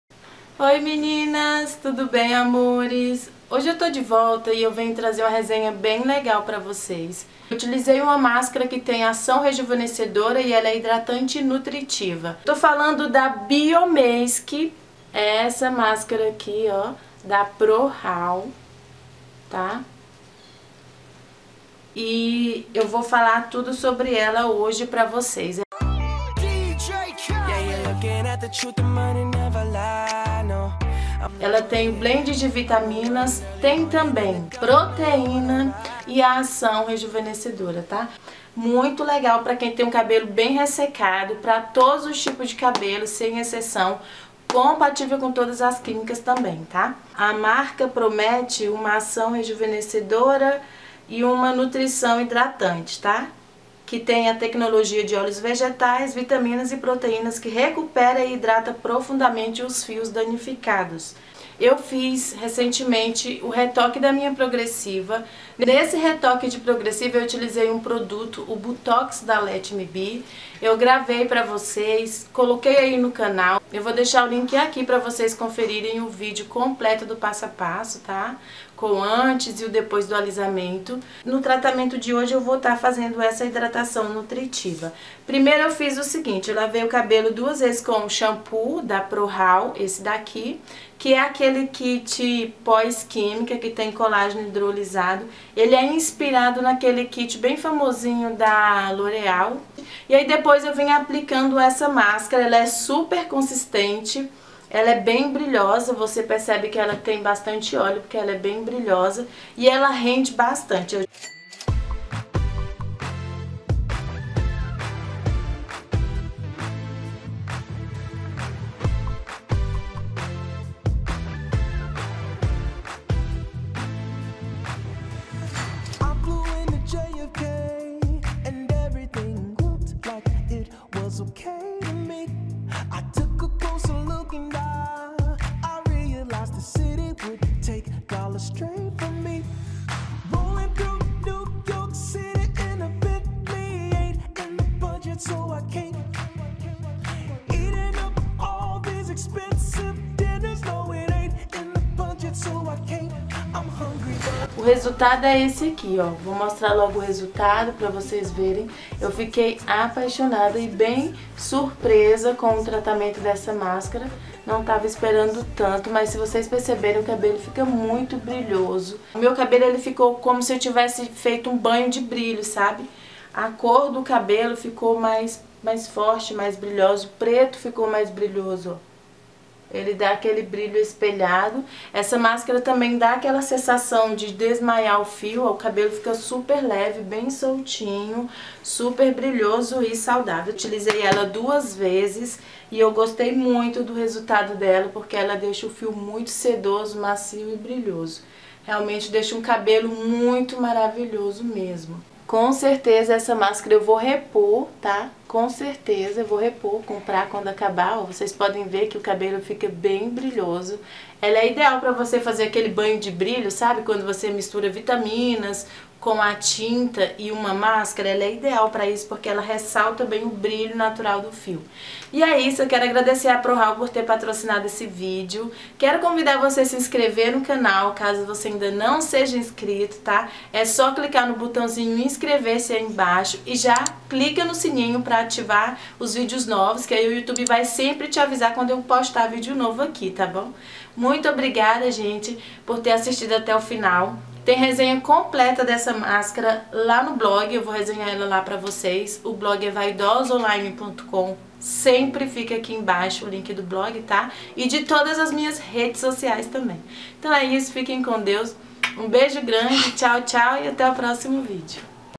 BIOMASK PROHALL COMO USAR
prohall cabelo prohallresenha Amores eu Testei a Máscara Biomask da Prohall. Ela contém óleo de coco a queratina e o ácido láctico, ela se encaixa nas 3 etapas do cronograma capilar e ainda é liberada para Low poo. Ela hidrata, nutri devido o óleo de coco, reconstrói por conter queratina, e sela a cutícula do fio devido a presença do ácido láctico e tem ação rejuvenescedora. Hidratação potente, como recupera cabelos ressecado, e ter cabelo liso de salão.

Gostou já se inscreve, curte e comenta ok bjs.

0:01 inscreva-se no canal ;-)
0:05 e ative o sino
AOSS1985
AOSS1985